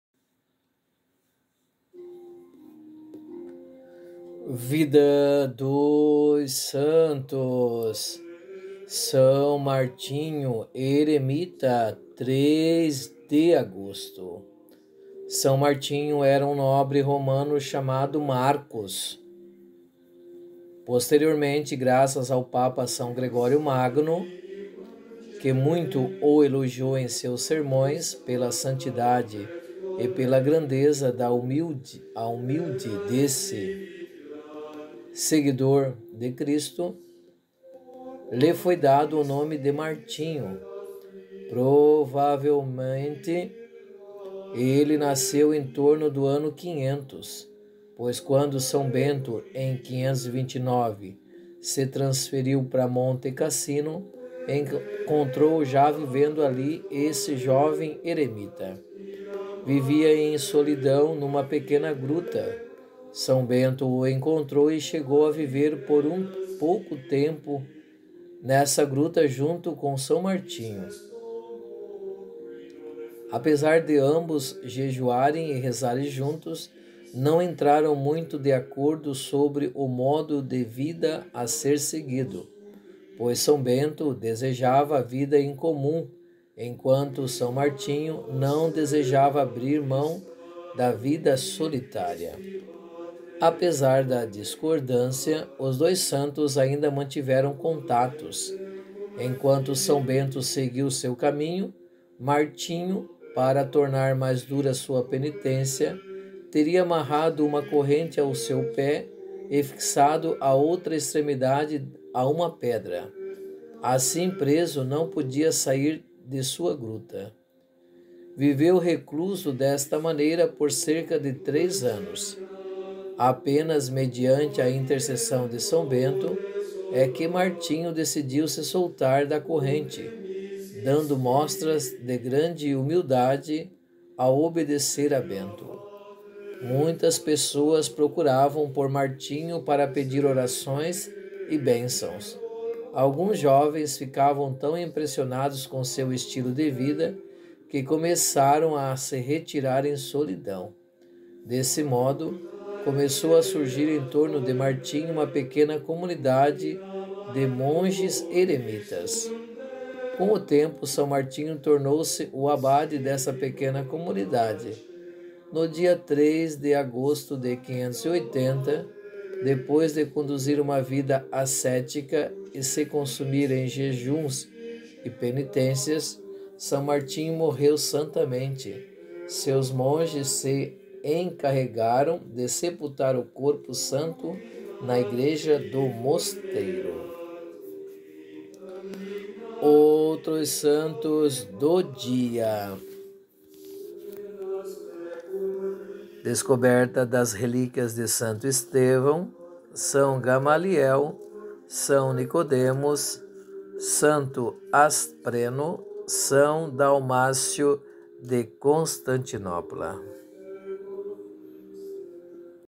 Vidas dos Santos - São Martinho Eremita 03 Ago
Viveu em contemplação e oração numa gruta do Monte Mássico, Itália. Quer ser mais íntimo de Nossa Senhora leia o Tratado da Verdadeira devoção e faça sua consagração. Acesse @eloiziototustuus